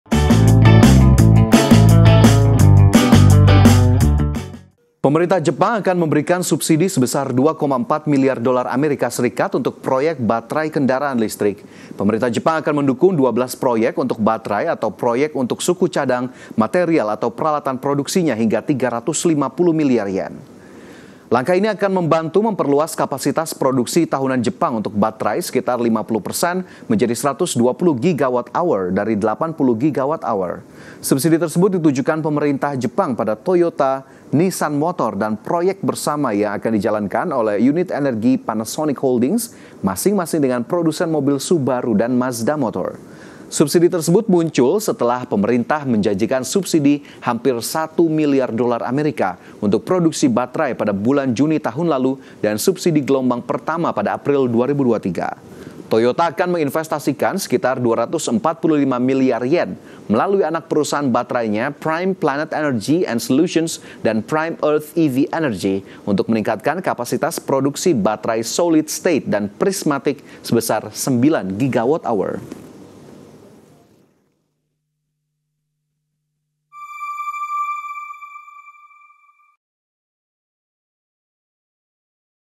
Jepang Berikan Subsidi USD2,4 Miliar untuk Proyek Baterai EV | IDX CHANNEL
Pemerintah Jepang akan memberikan subsidi sebesar 2,4 miliar dolar AS  untuk proyek baterai kendaraan listrik. Pemerintah Jepang akan mendukung 12 proyek untuk baterai atau proyek untuk suku cadang material atau peralatan produksinya hingga 350 miliar yen.